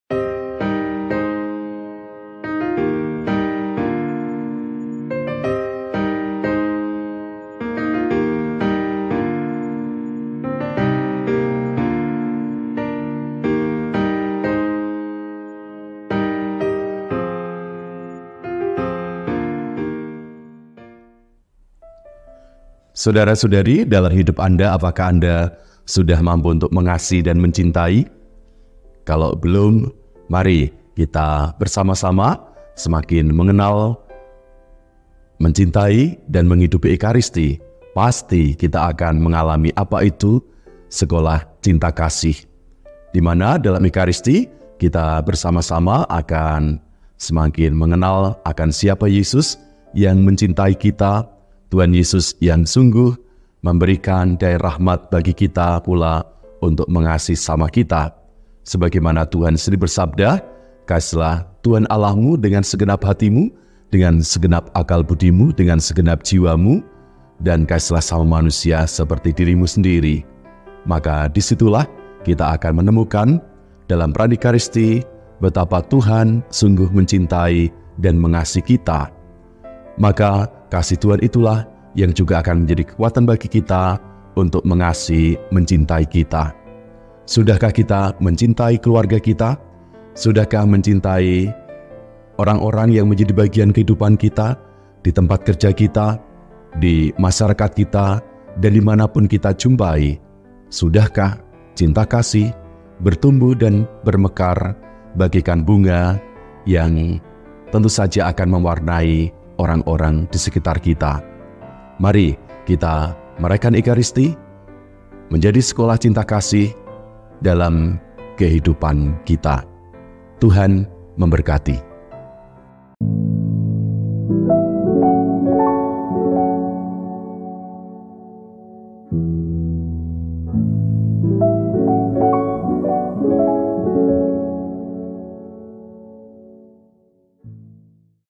MANNA - Makanan Perjalanan Episode 2, oleh : RD Ignatius Adam Suncoko
MANNA - Makanan Perjalanan

Episode 2,
EKARISTI SEKOLAH CINTA KASIH Pengantar  oleh :
 RD Ignatius Adam Suncoko

Rangkaian Katekese Visual, persembahan dari Bidang Pewartaan & Bidang Liturgi Paroki SPMDGK
Sudahkah kita mampu mengasihi dan mencintai ? Dengan mencintai dan menghidupi Ekaristi , kita pasti akan mengenal Sekolah Cinta Kasih